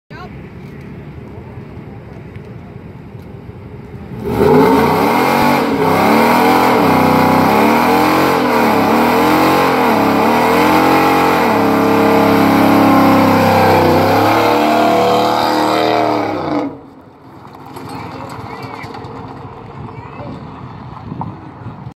Flint street racing 2018
Allan and the z28 camaro at our meet